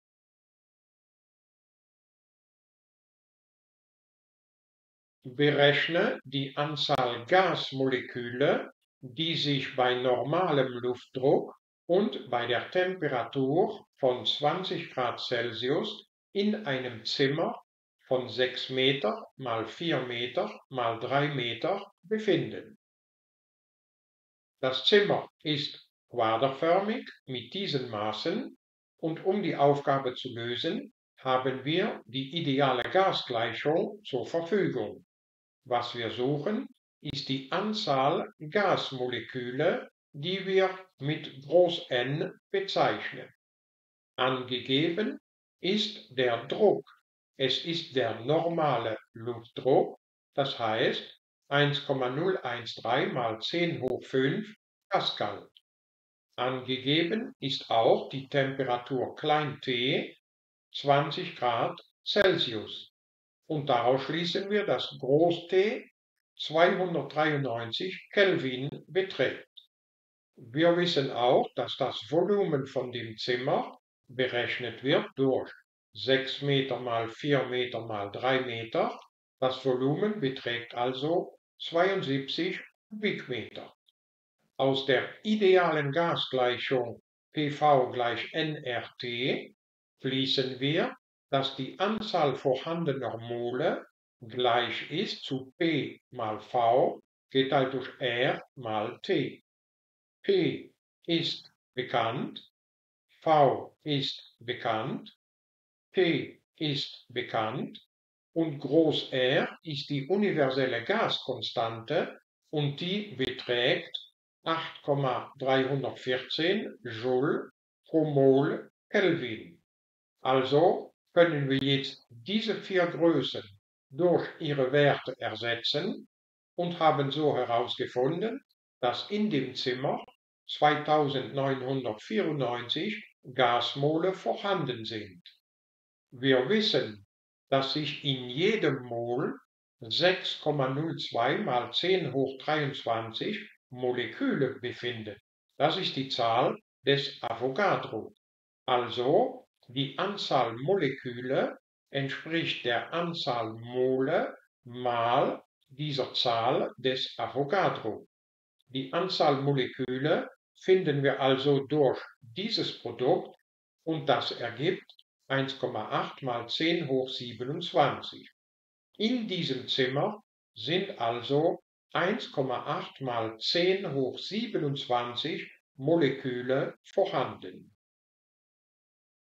Physik 6PhD022 Die ideale Gasgleichung
Durch die ideale Gasgleichung und die Zahl des Avogadro berechnen wir die Anzahl Gasmoleküle, die sich in einem Zimmer befinden.